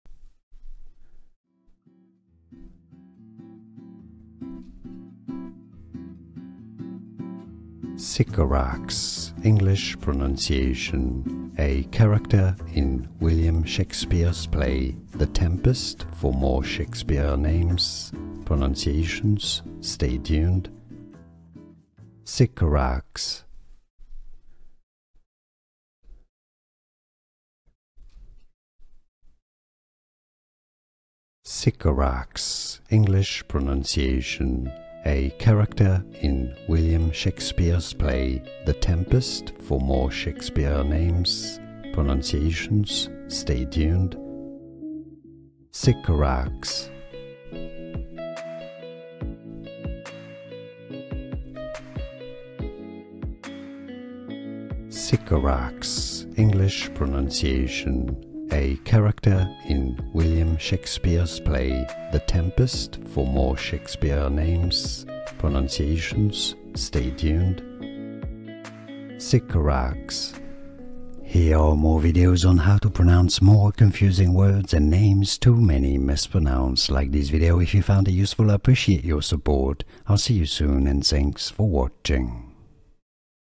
How to Pronounce Sycorax
This video shows you How to Pronounce Sycorax, pronunciation guide.

00:00 - Pronunciation Intro
00:16 - How to Pronounce Sycorax

How to Pronounce Sycorax. What does this word/name mean? Words’ meaning, dictionary definition, explanation.
Information & Source
Sycorax is an unseen character in William Shakespeare's play The Tempest. She is a vicious and powerful witch and the mother of Caliban, one of the few native inhabitants of the island on which Prospero, the hero of the play, is stranded. Wikipedia
Play: The Tempest
Played by: Claire Davenport
Movie: The Tempest
Character Created By: William Shakespeare